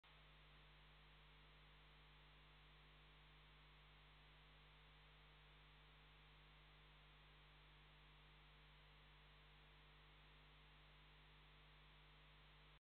Chouette effraie
Petite visite du nichoir à crécerelles par une chouette effraie